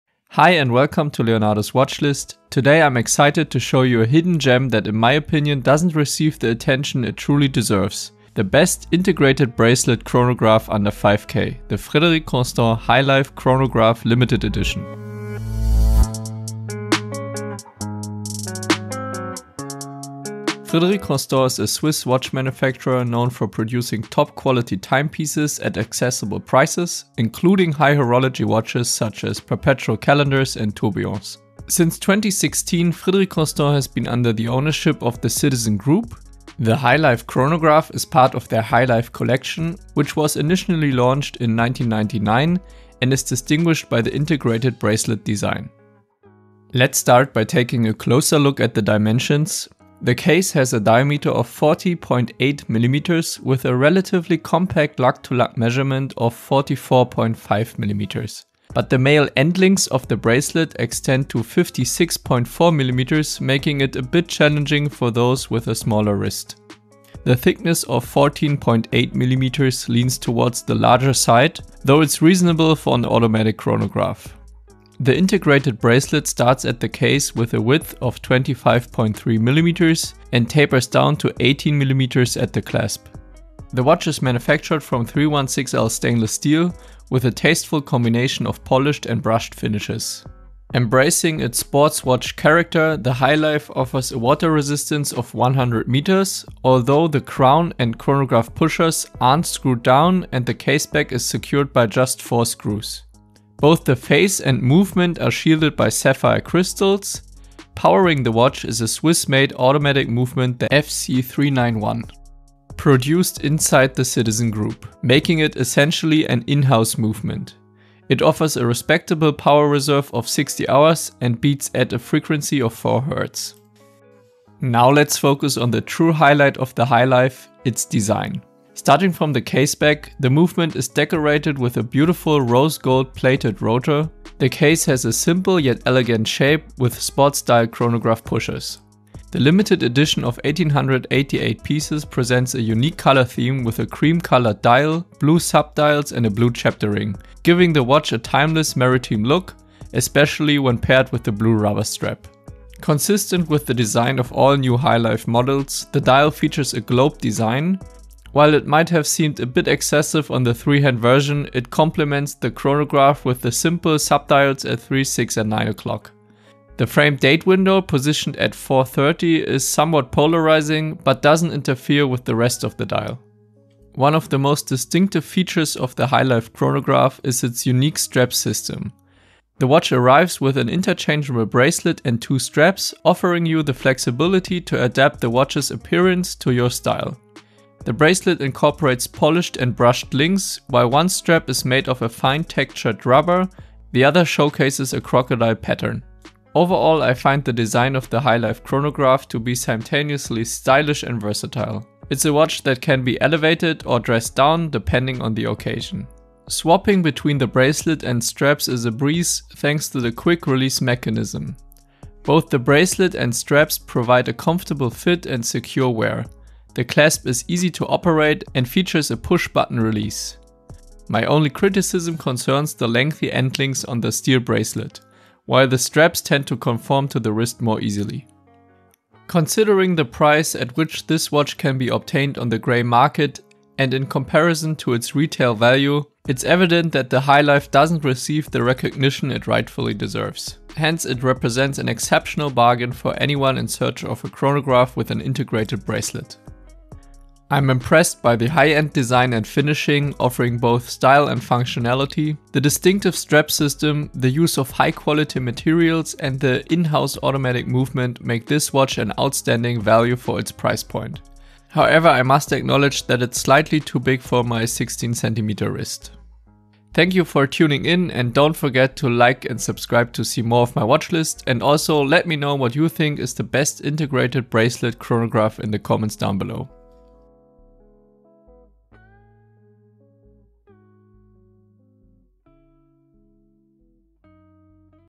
Review in 5 Minutes: Frederique Constant Highlife Chronograph
Review of my Frederique Constant Highlife Chronograph Limited Edition Ref. FC-391WN4NH6.